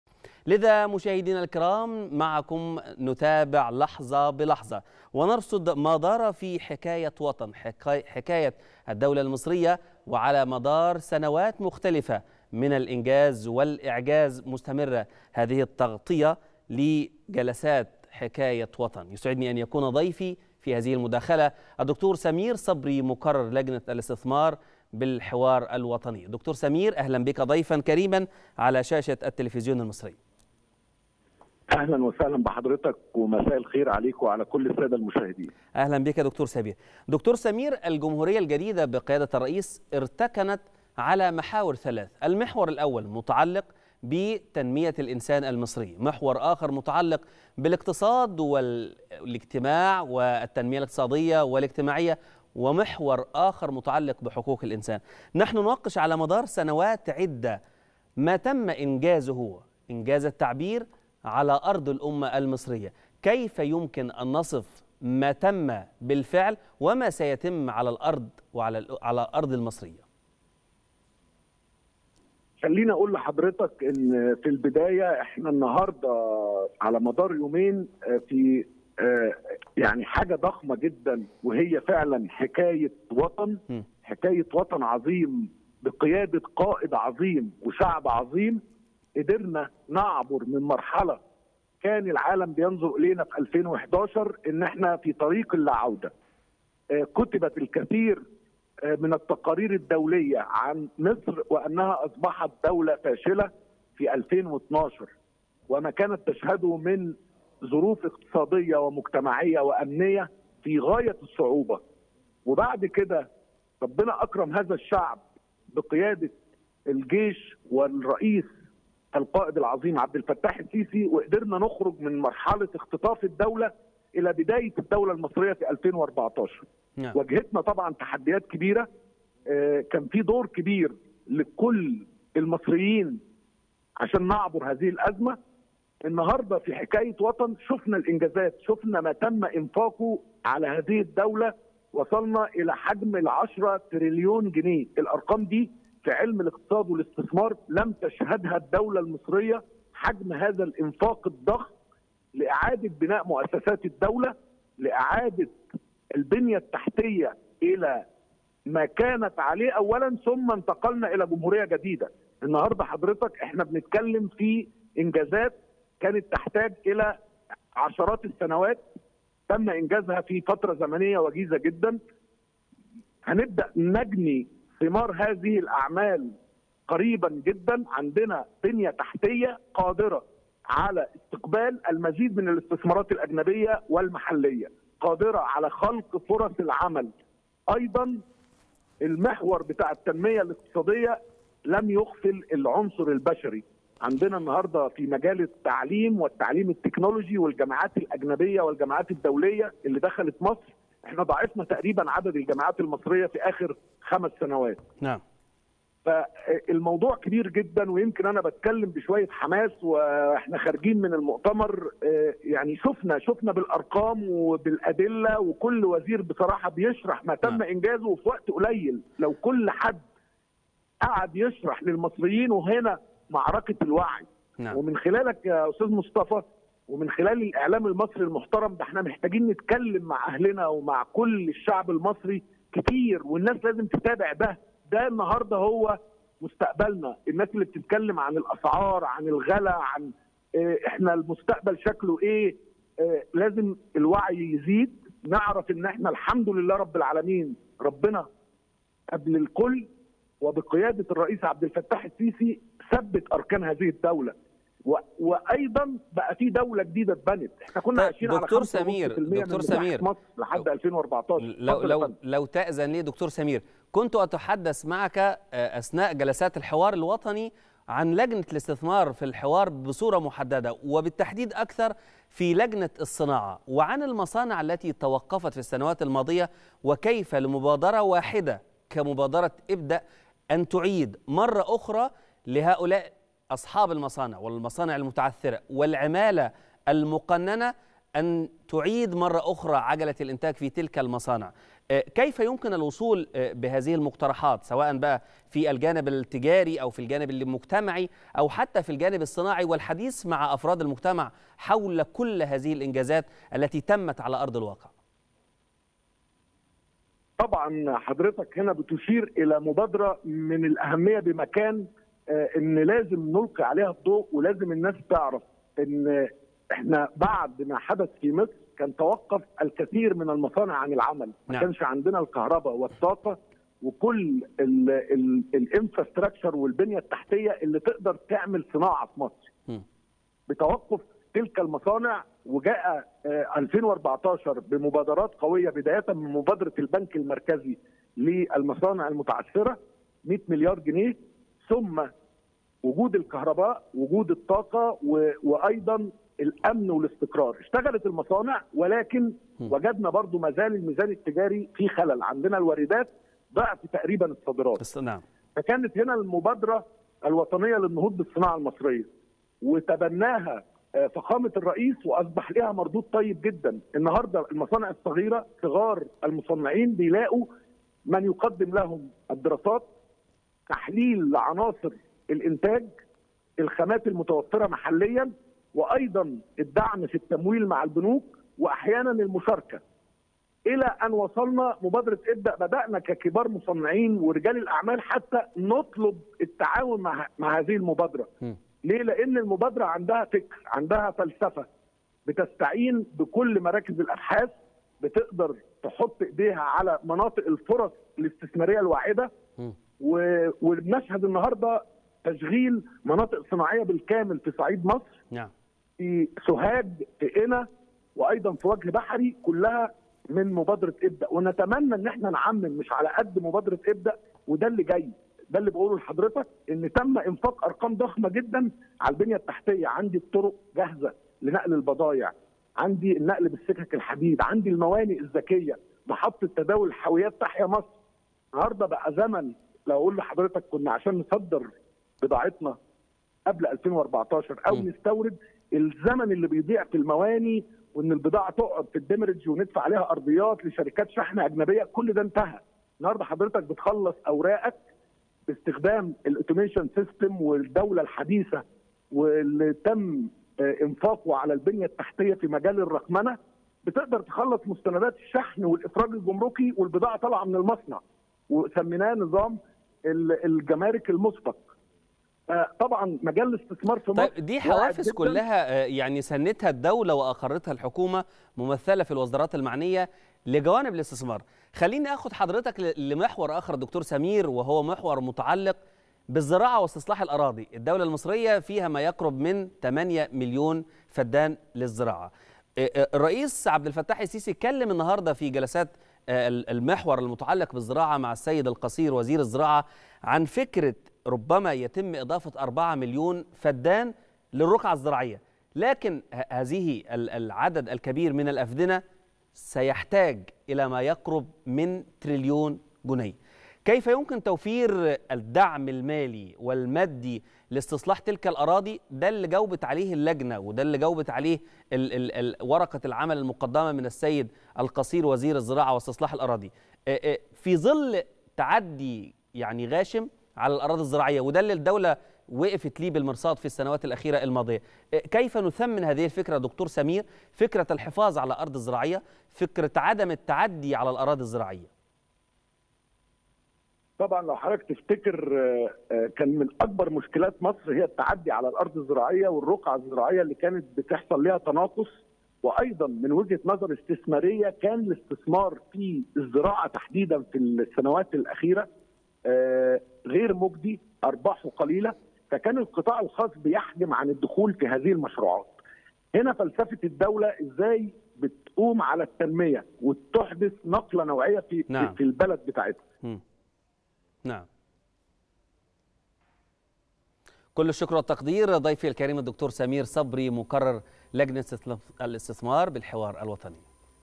د. سمير صبري : لدينا بنية تحتية قادرة على استقبال المزيد من الاستثمارات الأجنبية والمحلية
تغطية خاصة | د. سمير صبري مقرر لجنة الاستثمار بالحوار الوطني: لدينا بنية تحتية قادرة على استقبال المزيد من الاستثمارات الأجنبية والمحلية